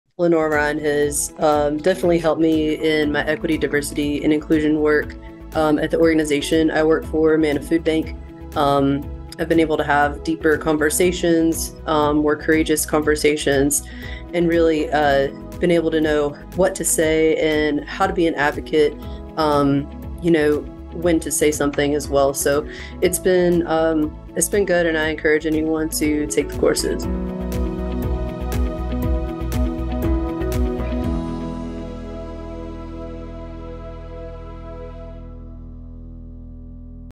The LREDI program: Ash Timmons' review
MANNA Food Bank's Equity, Diversity & Inclusion co-chair Ash Timmons talks about their experience with the Lenoir-Rhyne Equity & Diversity Institute (LREDI).